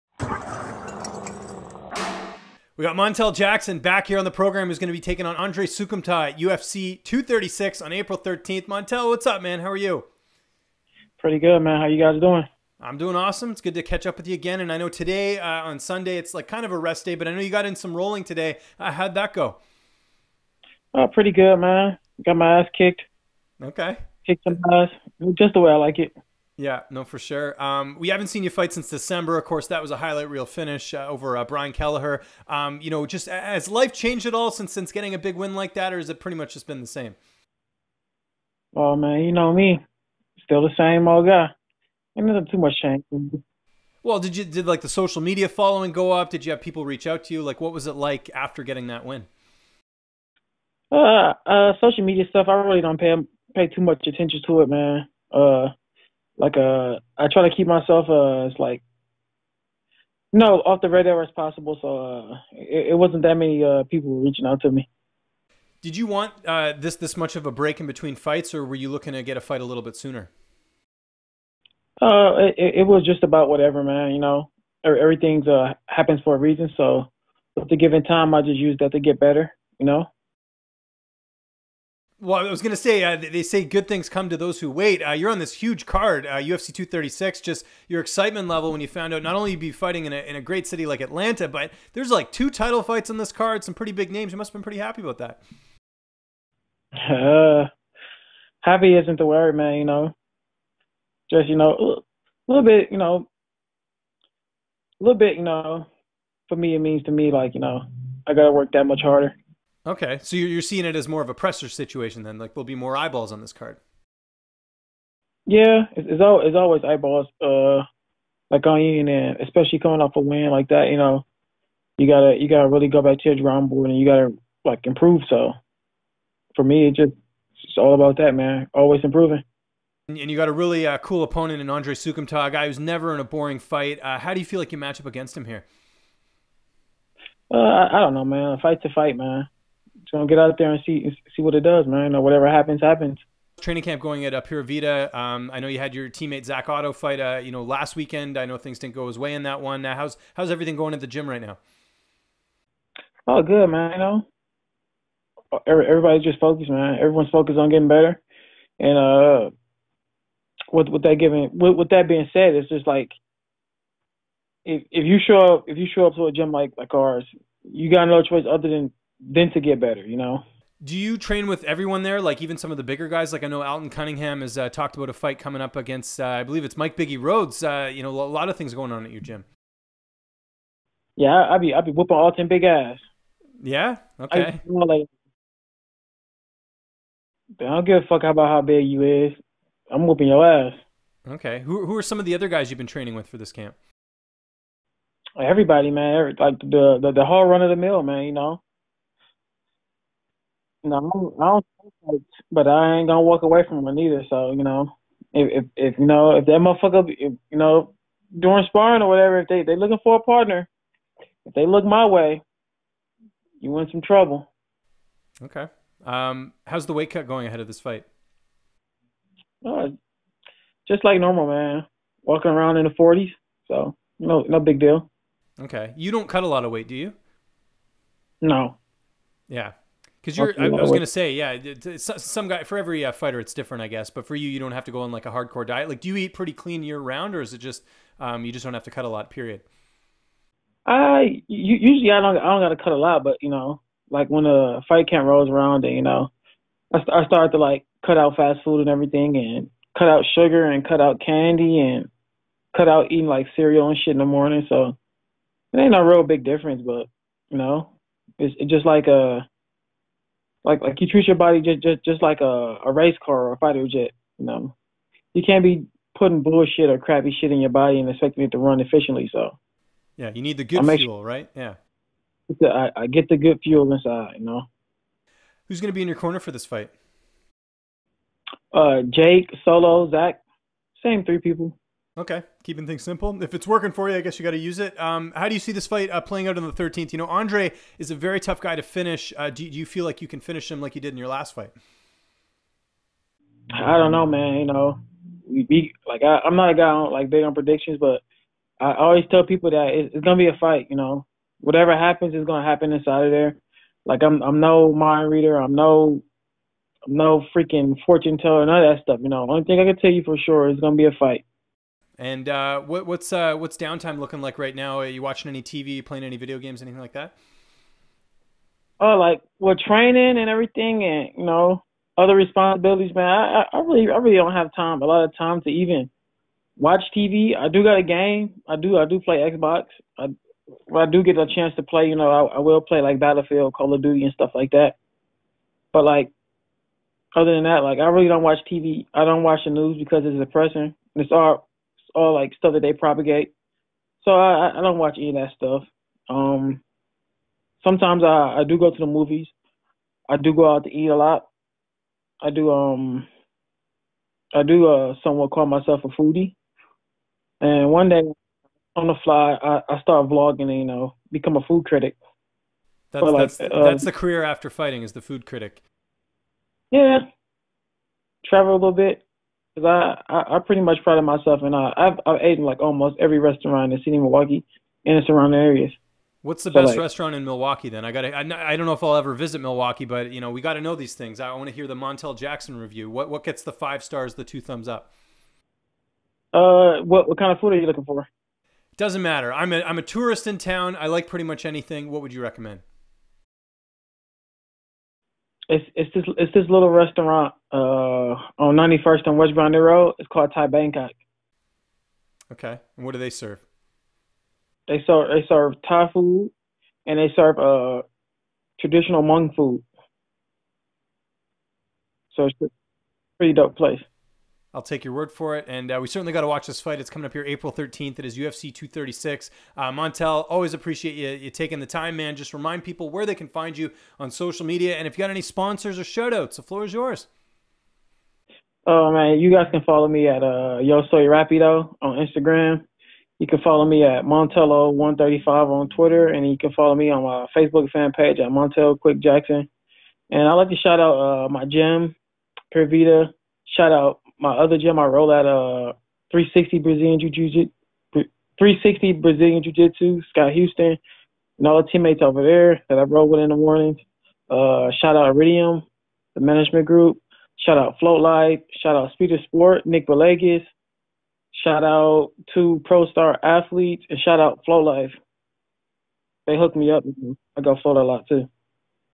UFC 236's Montel Jackson Looking To Ride the Momentum From His Last Fight
Montel Jackson (7-1) discusses his upcoming fight against Andre Soukhamthath (13-6) at UFC 236 on Apr. 13.